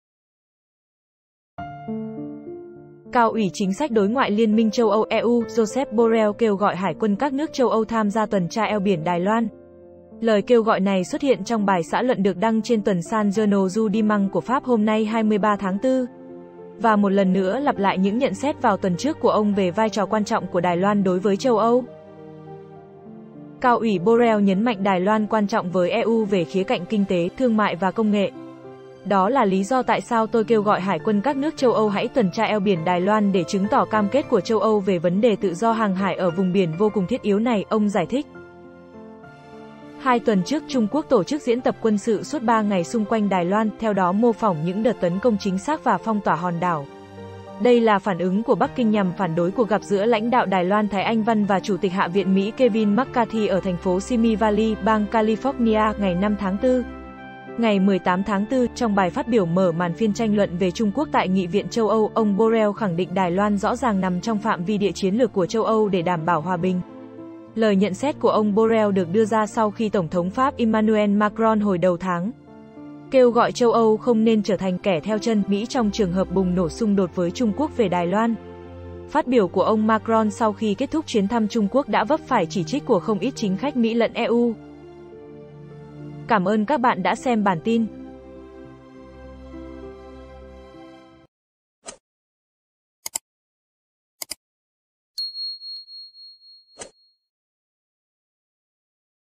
KHẢ NĂNG HẢI QUÂN CÁC NƯỚC CHÂU ÂU TUẦN TRA EO BIỂN ĐÀI LOAN
"Đó là lý do tại sao tôi kêu gọi hải quân các nước châu Âu hãy tuần tra eo biển Đài Loan để chứng tỏ cam kết của châu Âu về vấn đề tự do hàng hải ở vùng biển vô cùng thiết yếu này", ông giải thích.